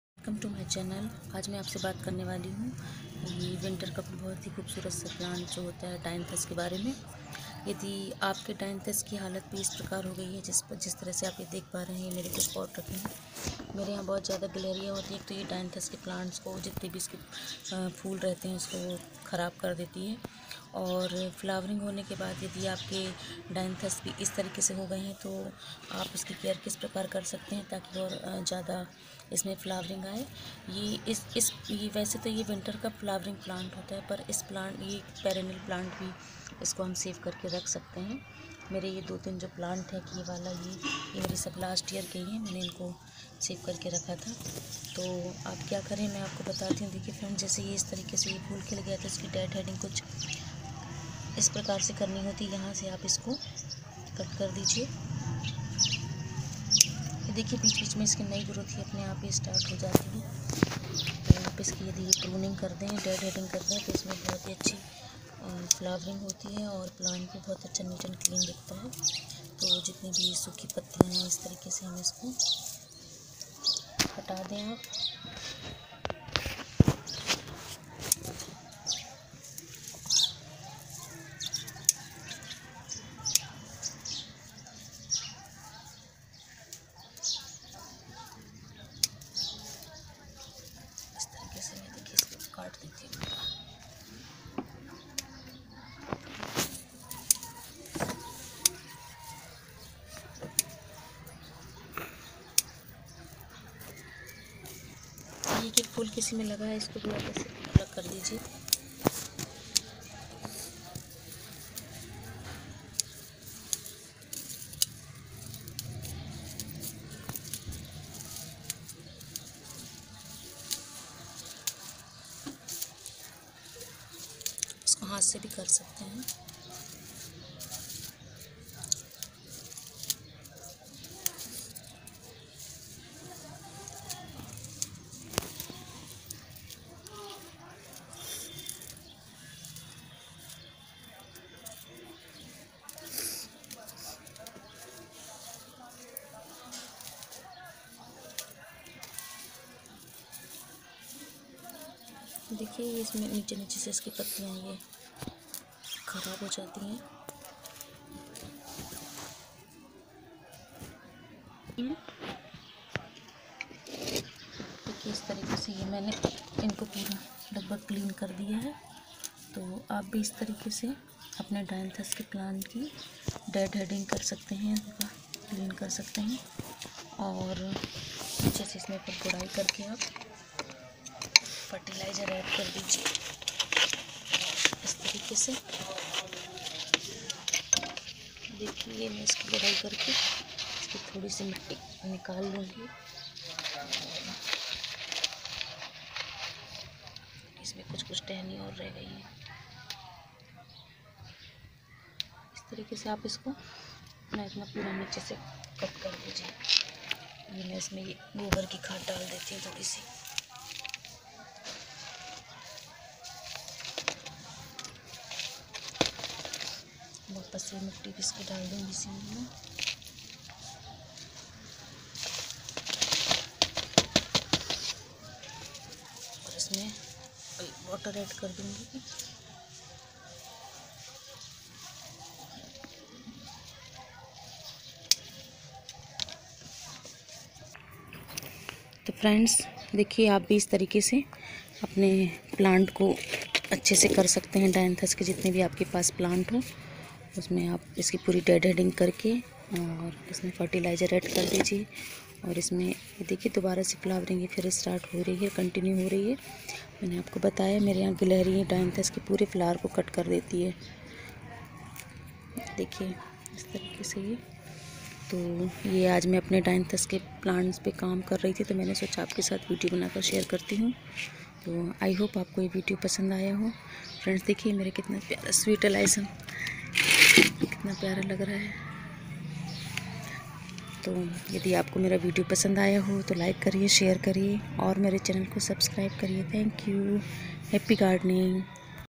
Dianthus plant care and tips || How to care dianthus plant......
Hello friends,

In today's video I will tell you about dianthus plant and it's care and tips